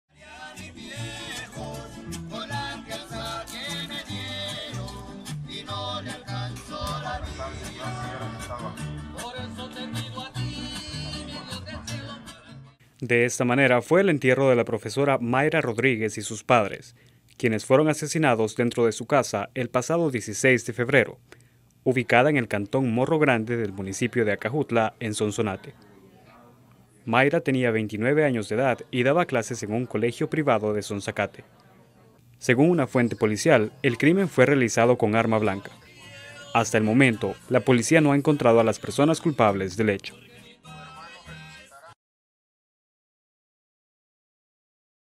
Entierran a familia asesinada dentro de su vivienda en Ahuachapán
Las víctimas fueron identificadas por las autoridades como Balmore Galileo Cerna de 60 años y Rosa Emilia de Cerna de 60, padres de la profesora Mayra Yesenia Rodríguez de 29 años, quien daba clases en el Liceo José Carmen Di Pietro, de Sonzacate, Sonsonate.

¡Visítenos!